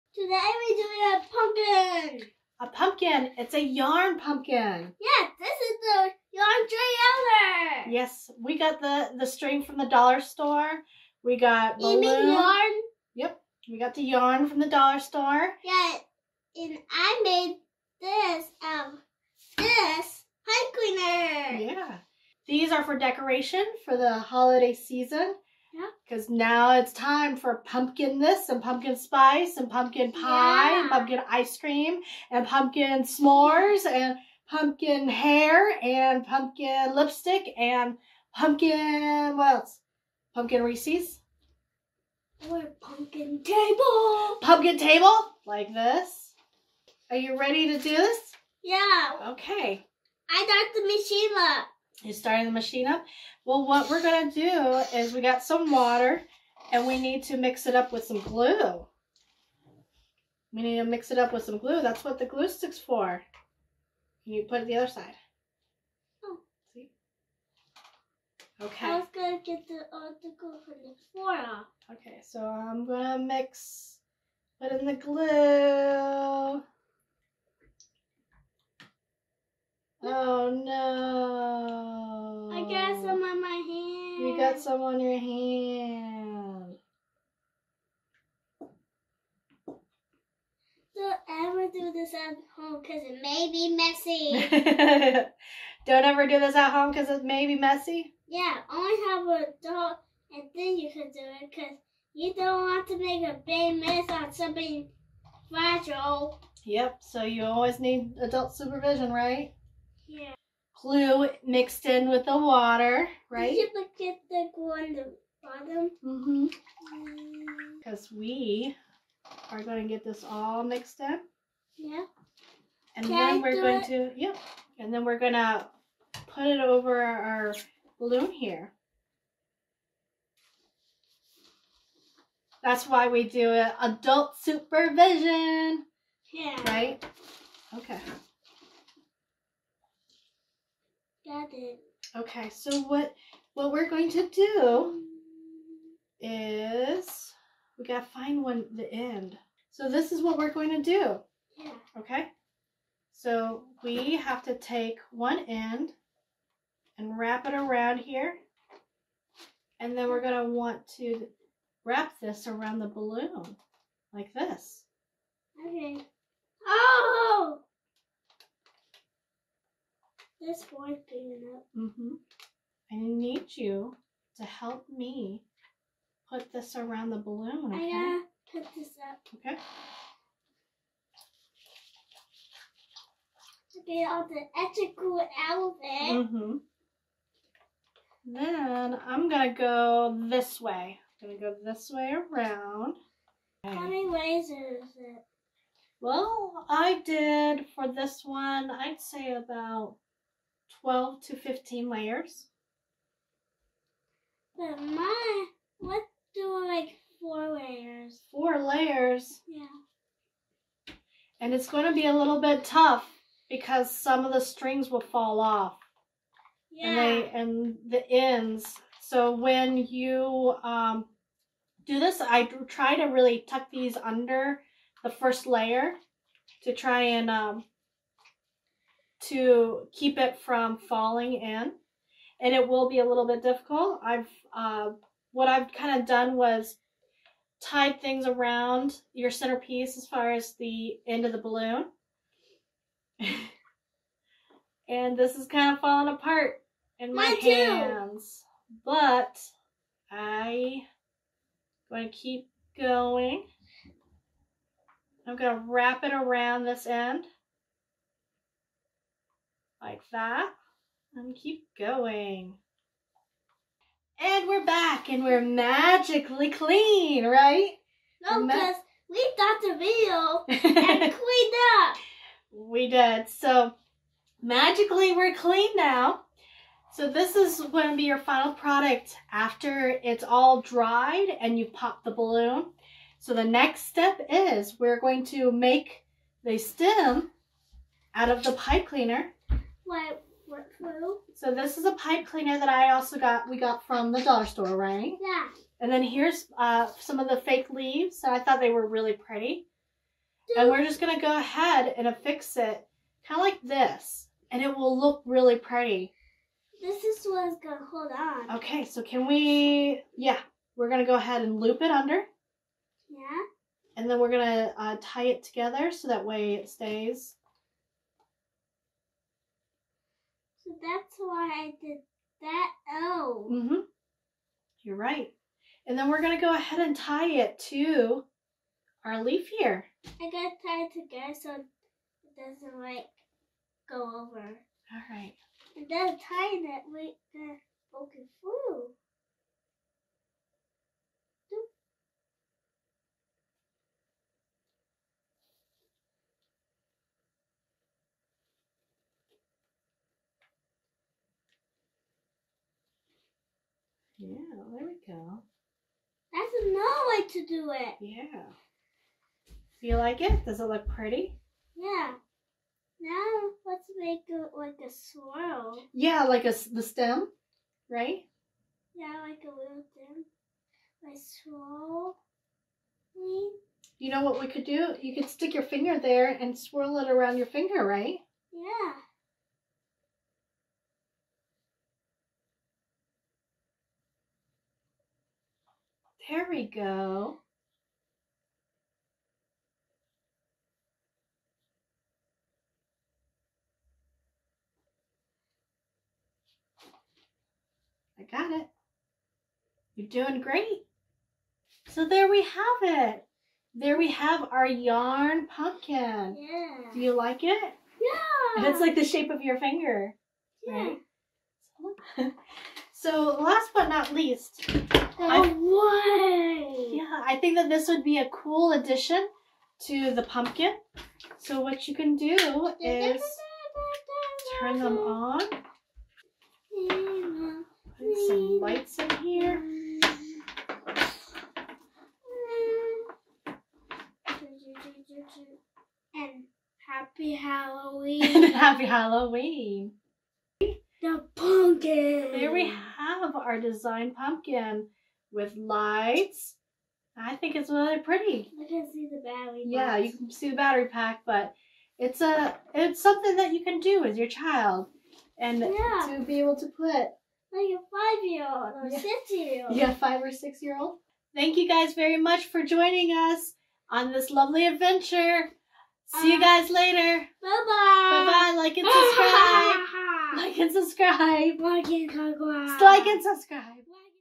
How to Make Yarn Pumpkins - Fun Children's Art and Craft for Halloween or Thanksgiving
In this children-friendly art and craft video, we show you how to make adorable yarn pumpkins using just some yarn, glue, and balloons. This is the perfect activity for Halloween or Thanksgiving, and is great for kids of all ages. Follow along as we guide you through the process step-by-step, and watch as your children's creativity and imagination take shape with each colorful pumpkin they create. So gather up some supplies and get ready to make some festive and fun decorations for your home!